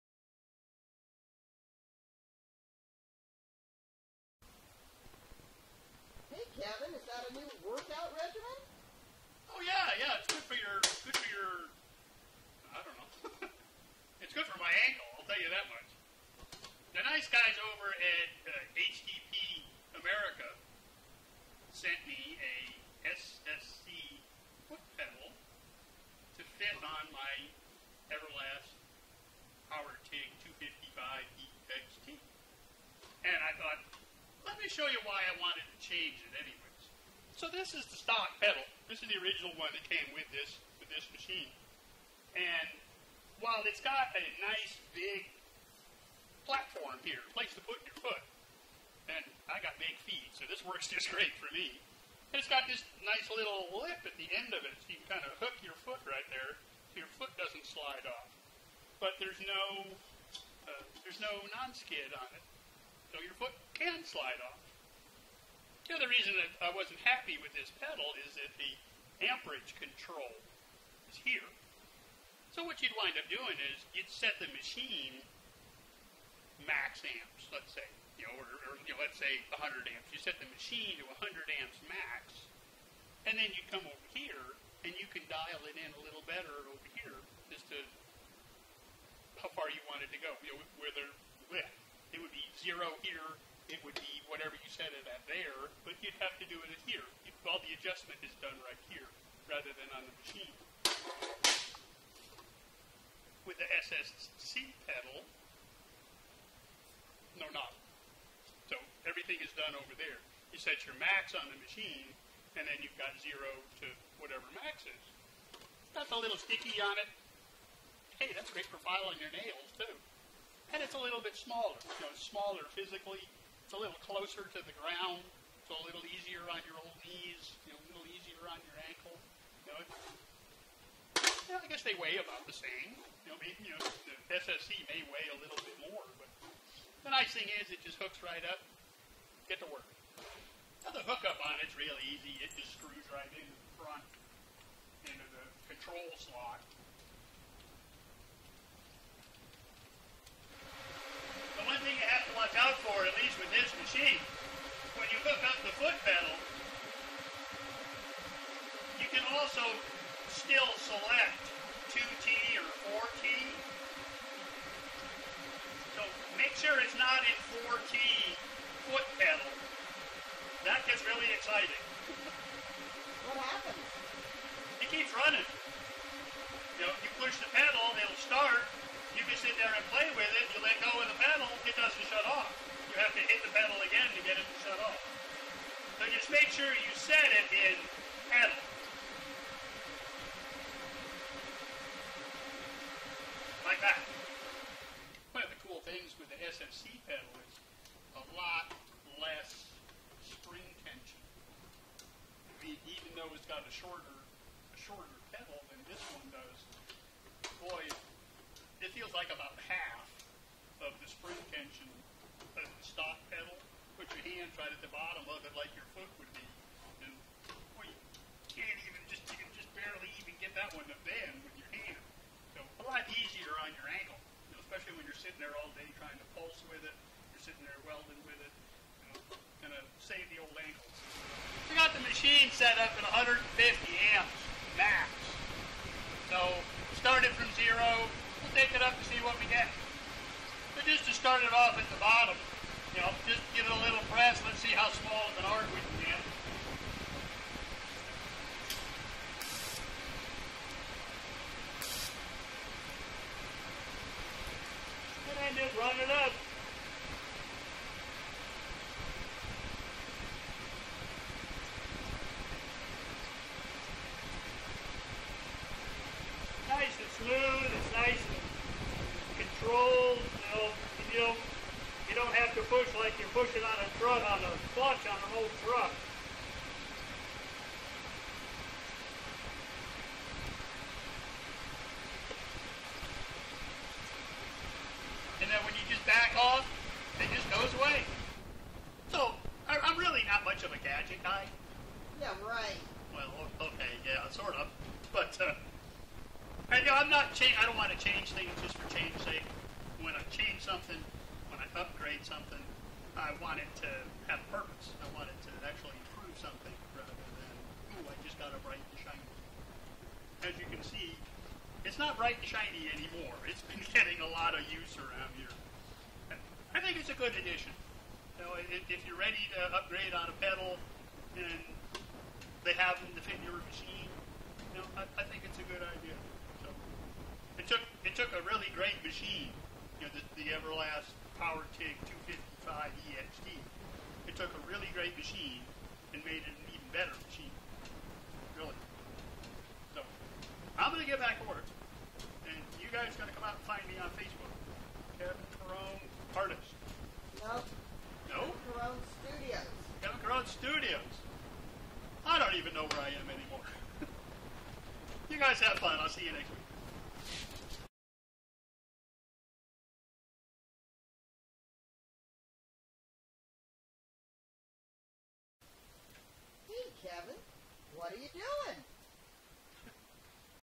Why I Replaced the Foot Pedal on My Everlast POWERTIG 255EXT - Kevin Caron
- Artist Kevin Caron explains why he put a new foot pedal on his Everlast 255EXT TIG welder - and shows how to do it .

Caron has two foot pedals on his workbench, the one that came with Everlast's POWERTIG 255EXT and a new SSC foot pedal from HTP America.

First Caron shows the stock pedal. It has a nice big platform for your foot and a nice lip to hook your foot on so it doesn't slide off, but there's no non-skid surface on it. The other thing he didn't like about the pedal that came with the machine is that the amperage control is on the back of the foot pedal itself. So you'd set the welder at, say, maximum amperage or 100 amps, then dial it in on the foot pedal. All the adjustment is done on the pedal.

On the SSC pedal, there is no knob. You set your welder at max amps and then control the amount of amperage with your foot pressure. The SSC foot pedal also has nonskid on the platform, which Caron likes.

The SSC pedal is a little smaller, too, and it's closer to the ground, which is a little easier on the knee and ankle. The two pedals weigh about the same, although the SCC pedal may weigh a little more.

The new pedal hooks up to the machine easily - it just screws right into the control receptacle on the front of the welder.

You do need to be careful to set the machine to "foot pedal." Be sure it's not in 4T,  because the welder will keep running unless you hit the pedal again to turn it off. So make sure you set the machine on "foot pedal."

Another nice thing about the SSC pedal is that it has less spring tension - maybe half as much - of the stock pedal. Again, that's a lot easier on your ankle.

Caron sets the welder at 150 amps and then gives the new foot pedal a test drive. He starts the arc small and then runs it up, and backs it down. It's nice and smooth and  controlled - you don't have to push on it like you would the clutch of an old truck!

As for getting a new pedal, Caron says that, when he changes something, he wants it to have value, not just be a "bright and shiny." He looks at the pedal and says, "As you can see, it's not bright and shiny anymore - it's been getting a lot of use."

If you're ready to upgrade your foot pedal and they have one to fit your machine, Caron says it's a good idea. He says it took a really great machine - the Everlast POWERTIG 255EXT - and made it an even better one.

Caron is ready to go back to work, so you have time to head out to see more how-to videos, or come out and "like" his Facebook and join the fun there.

(Well, you might want to stick around for another minute to see Caron dance .)